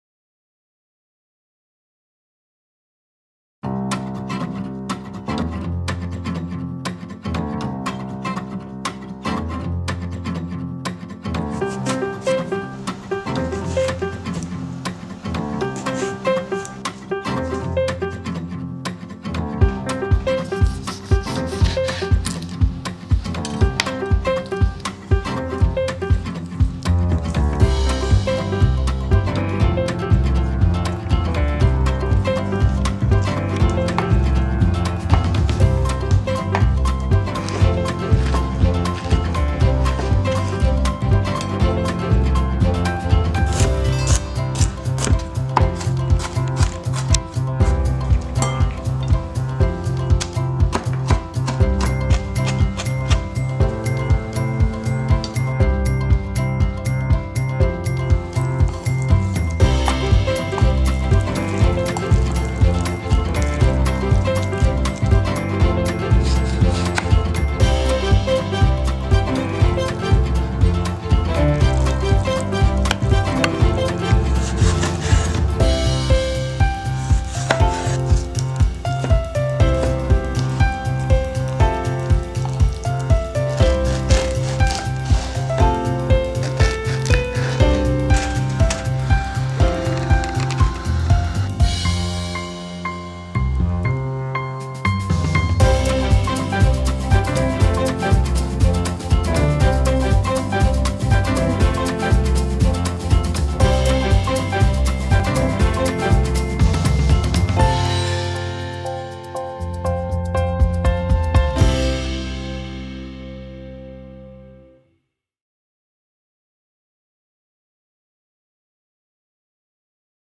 Smoky Eggplant Bruschetta
Delicious Smoky Eggplant Bruschetta makes for perfect starter or appetizer to any meal or party!

FOLLOW DISHES & SUCH!!!